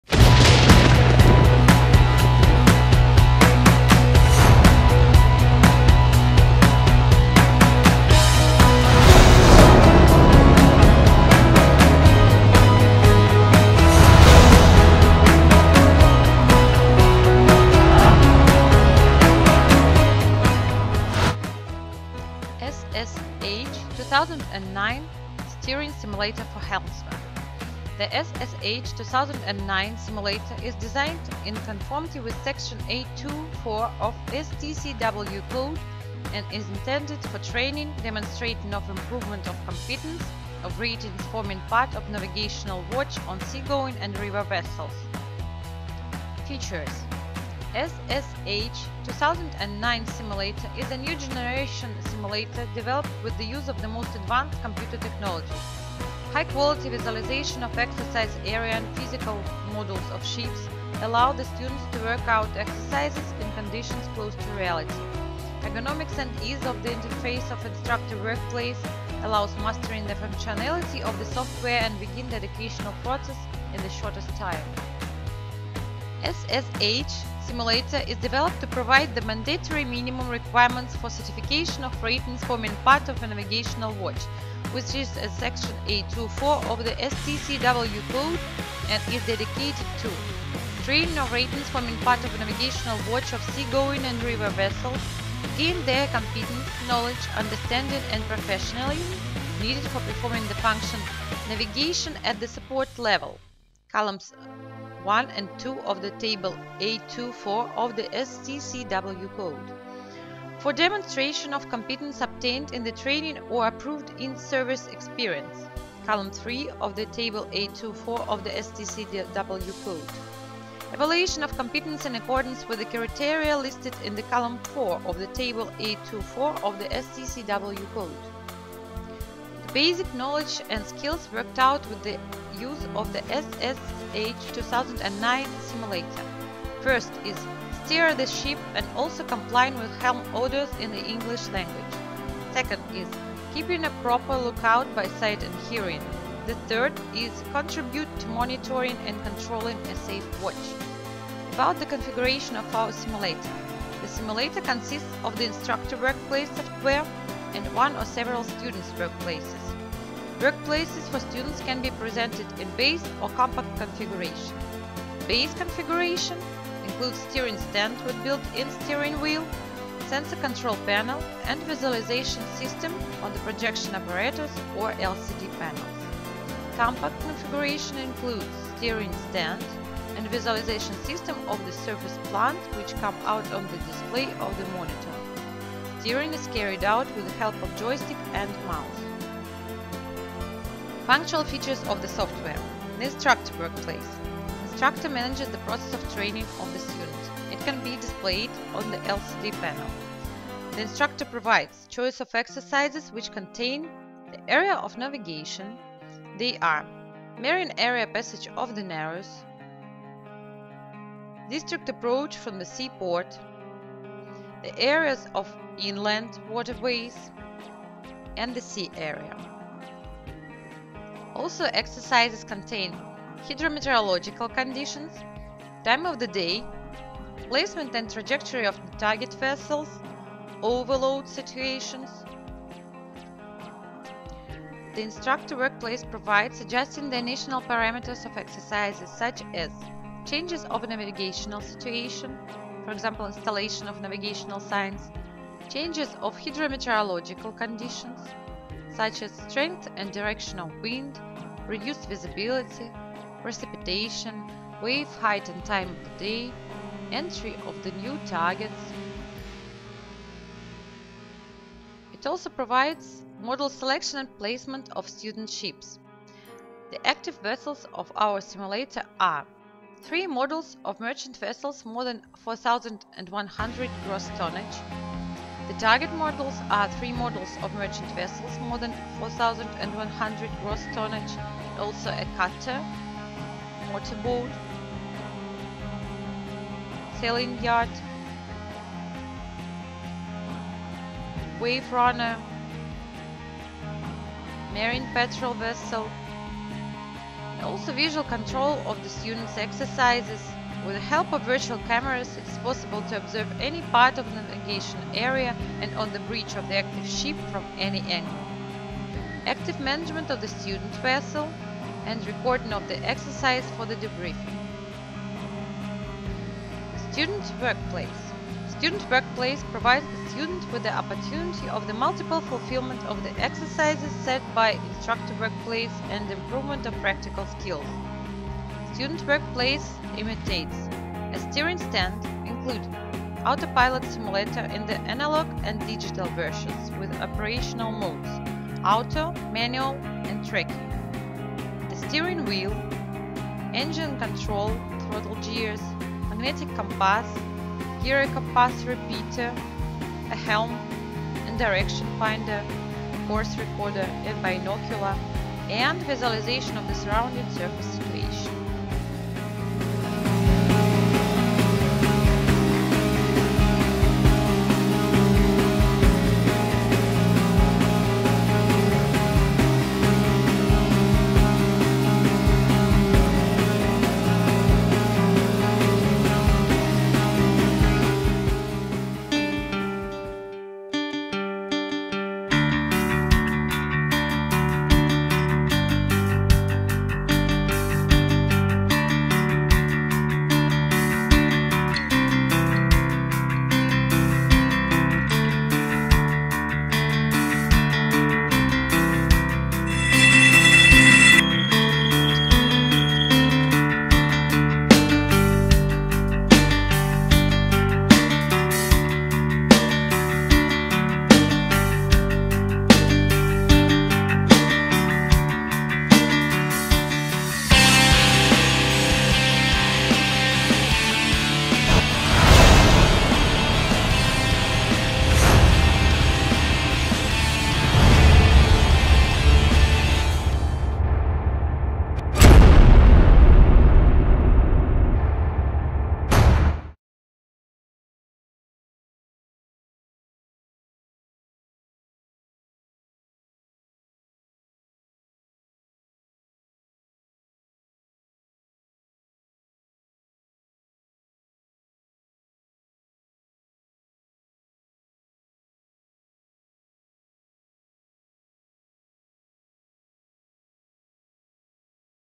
SSH-2009 Steering Simulator for helmsman
The "SSH-2009" simulator is designed in conformity with Section А-II/4 of STCW Code and is intended for training, demonstrating of improvement of competence of ratings forming part of a navigational watch on sea-going and river vessels.